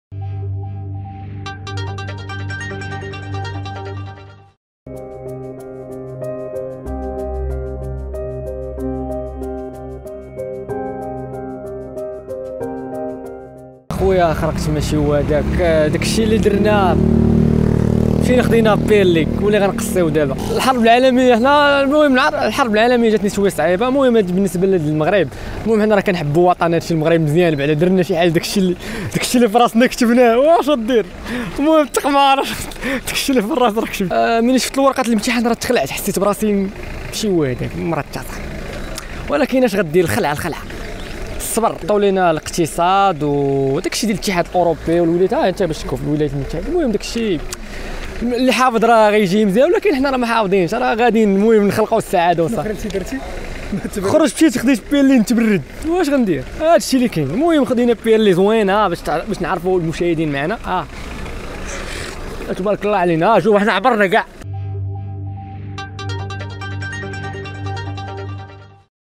مول "بيرلي" دخل يدوز الامتحان وتصدم:شفت ورقة الامتحان وتخلعت وشريت بيرلي باش نتبرد
بشكل طريف، كشف شاب من مدينة الدار البيضاء عن صدمته من صعوبة امتحان مادة الاجتماعيات